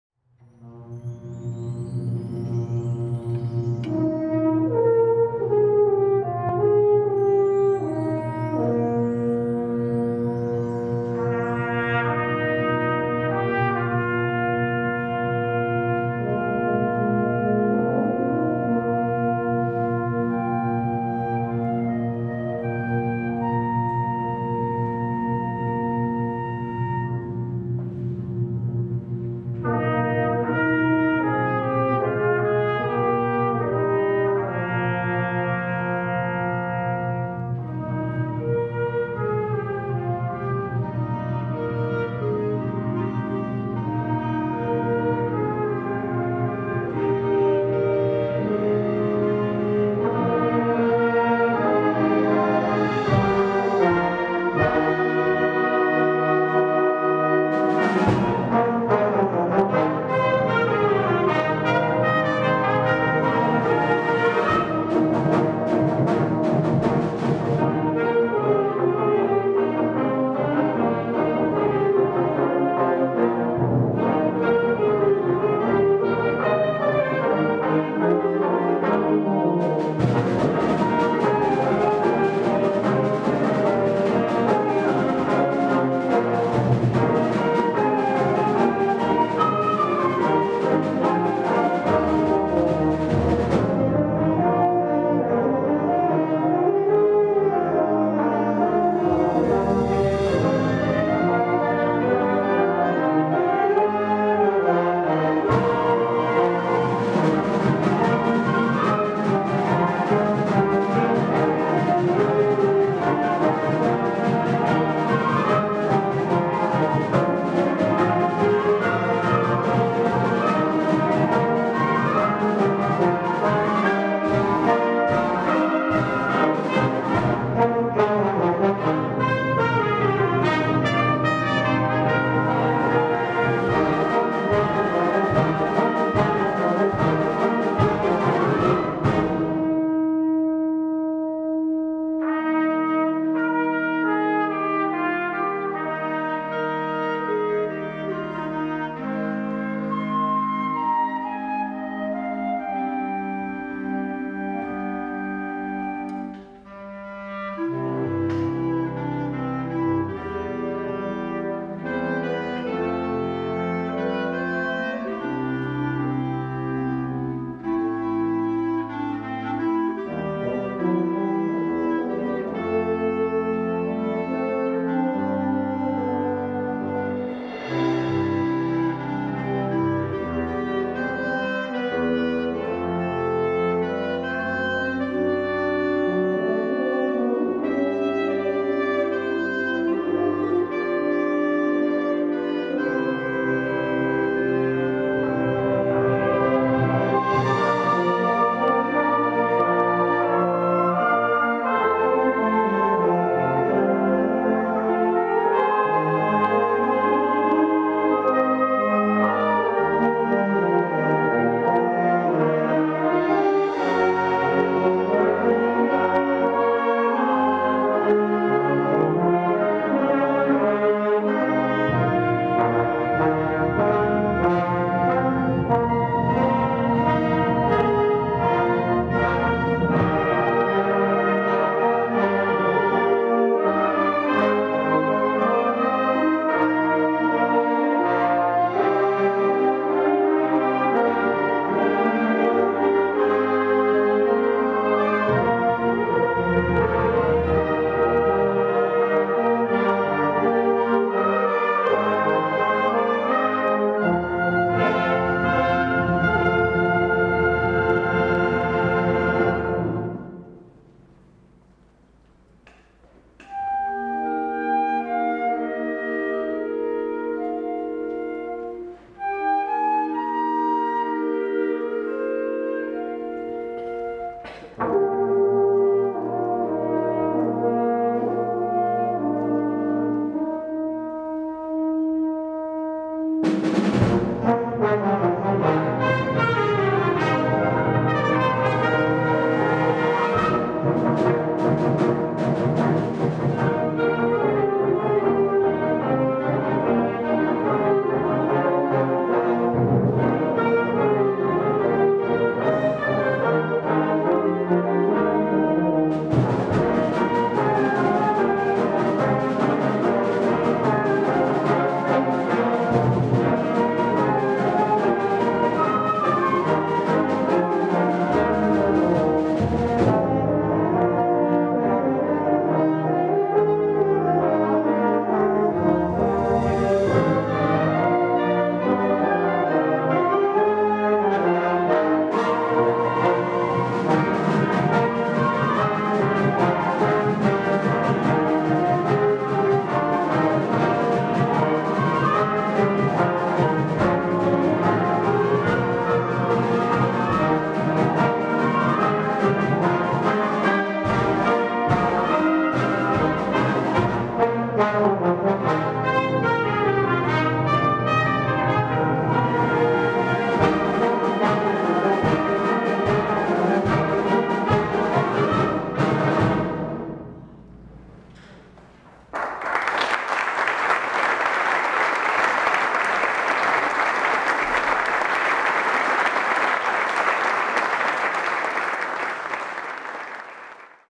Der Musikverein Oberglatt am Zürcher Unterländer Musiktag 2022.
Der Musikverein Oberglatt spielt am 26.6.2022 im Gemeindehaussaal in Embrach.